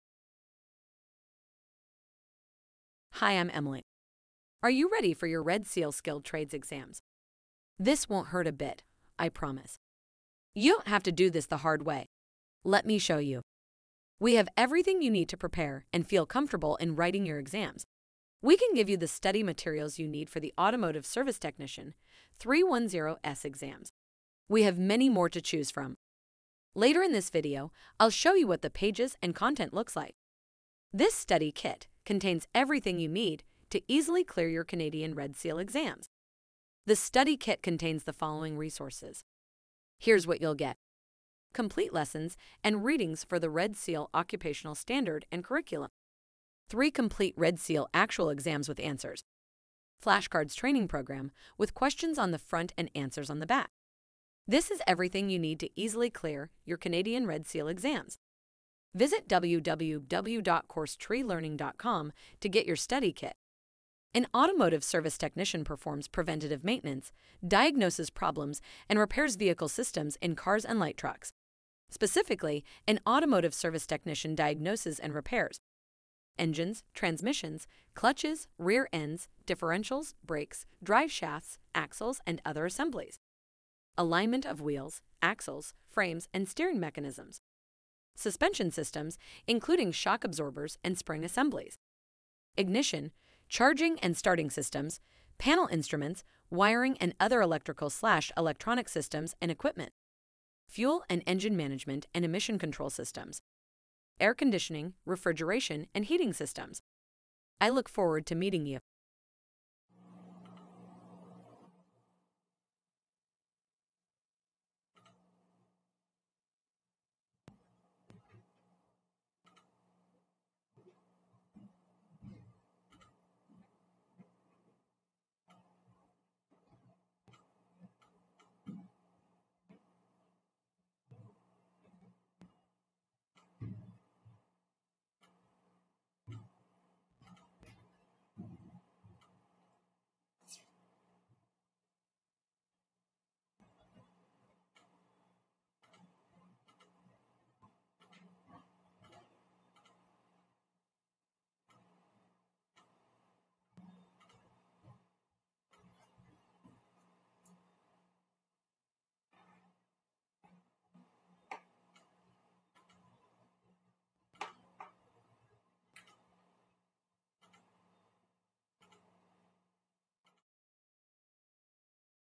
Automotive Service Technician Red Seal 310S Exam Prep Kit Textbook Readings, Exam Questions PDF
Automotive Service Technician Red Seal 310S Exam Prep Kit

The Study Kit Contains the Following Resources:

Complete lessons and readings for the Red Seal Occupational Standard and curriculum.

Complete Red Seal actual exams with answers.

Flashcards training program, with questions on the front and answers on the back.

This is everything you need to easily clear, your Canadian Red Seal exams.

Automotive Service Technician

An Automotive Service Technician performs preventative maintenance, diagnoses problems and repairs vehicle systems in cars and light trucks. Specifically, an Automotive Service Technician diagnoses and repairs:

engines, transmissions, clutches, rear ends, differentials, brakes, drive shafts, axles and other assemblies alignment of wheels, axles, frames and steering mechanisms suspension systems, including shock absorbers and spring assemblies ignition, charging and starting systems, panel instruments, wiring and other electrical/electronic systems and equipment
fuel and engine management and emission control systems air-conditioning, refrigeration and heating systems